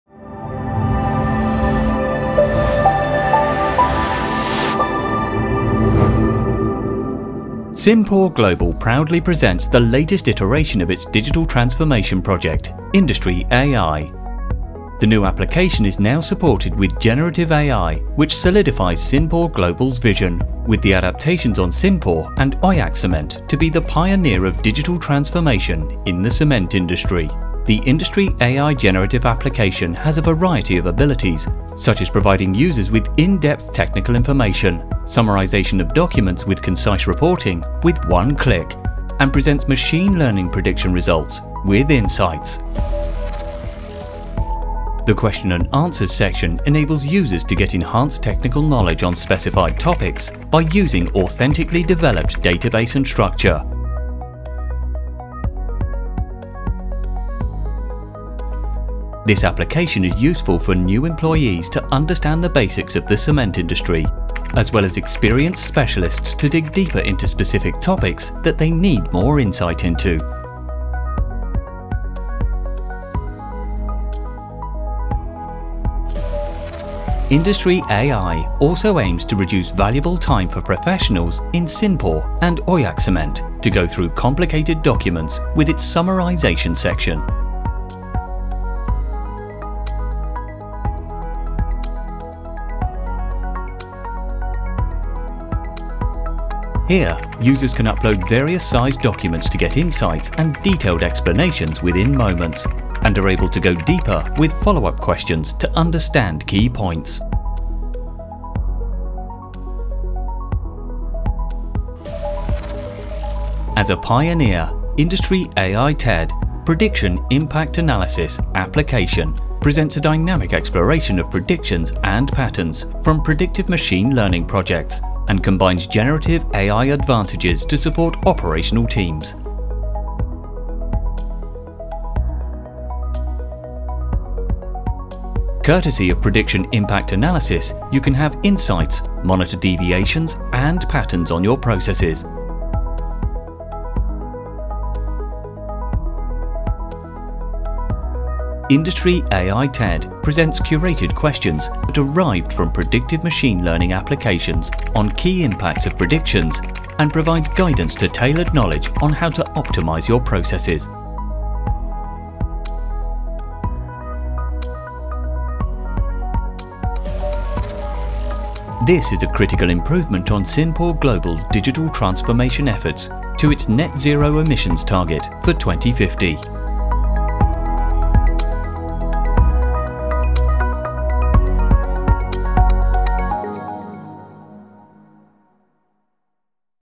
CIMPOR Global, IndustrAI'ın yeni sürümünü gururla sunar
CIMPOR Global, dijital dönüşümün öncüsü olma vizyonunu yeni versiyonu ile üretken Yapay Zekâ desteği sağlanan IndustrAI ile güçlendiriyor. Çimento sektöründe devrim yaratacak olan bu yeni sürüm, kullanıcılarına benzersiz teknik bilgi sağlama, belgeleri özetleme ve makine öğrenimi tahmin sonuçlarını içgörülerle sunma gibi birçok yenilikçi yeteneği bir arada sunuyor.

IndustrAI'in Gücünü Serbest Bırakın:

• Soru & Cevap Bölümü: Zengin içerik ve özgün veri tabanı ile eğitilmiş olan bu bölüm, sektöre yeni giren ve süreçleri anlamaya çalışanlar için olduğu kadar, deneyimli uzmanların daha fazla teknik bilgi ihtiyacını karşılamak için de güçlü bir araçtır.

• Summarization (Özetleme) Bölümü: CIMPOR çalışanlarının karmaşık belgeleri incelemek için harcadıkları zamanı önemli ölçüde azaltır. Kullanıcılar, belgeleri yükleyerek hızlıca dosyanın özetine ulaşabilir ve anahtar noktaları anlamak için tamamlayıcı sorularla daha derin bir öğrenim sağlayabilirler.

• TED (Tahmin Etki Değerlendirmesi) Bölümü: Sektörde öncü rol oynayan CIMPOR Global, bu bölüm ile tahmine dayalı makine öğrenimi projelerinden elde edilen tahminlerin ve modellerin dinamik bir şekilde incelenmesine yardımcı olur. Saha ekipleri, üretken yapay zekânın avantajları ile desteklenir. TED, tahminlerin temel etkileri üzerine makine öğrenimi uygulamasından türetilen seçilmiş soruları sunarak, süreçlerin optimize edilmesine yönelik özel bilgiler sağlar. Bu sayede, öngörülerinizi değerlendirebilir, sapmaları ve eğilimleri takip edebilirsiniz.

Net-Zero Hedefine Doğru Adım Adım

CIMPOR Global, 2050 Net-Sıfır emisyon taahhüdümüze yönelik cesur adımlar atarak yenilikçi üretken zeka uygulamaları ile geleceğin fabrikalarını ve iş gücünü şekillendiriyoruz. IndustrAI'ın yeni versiyonu ile çimento teknolojisindeki liderliğimizi pekiştiriyor ve inovasyon öncüsü konumumuzu yeniden kanıtlıyoruz.